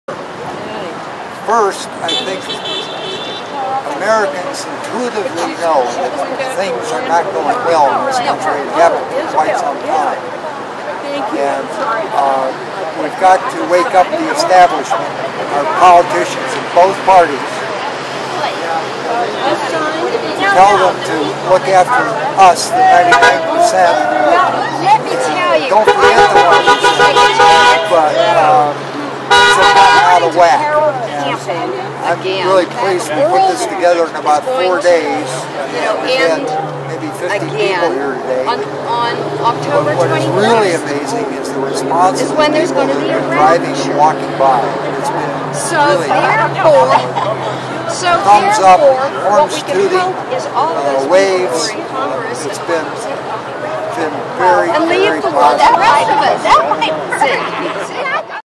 Doug Freeman, Former Chairman of Henderson County, NC Democratic Party
Doug tells why he believes we need to gather in support of Occupy Wall Street.  Unfortunately, the enthusiastic chatter of demonstrators and cars honking in support as they drive by, make it a bit difficult to hear him.  Repeat events are scheduled for the next three Saturdays.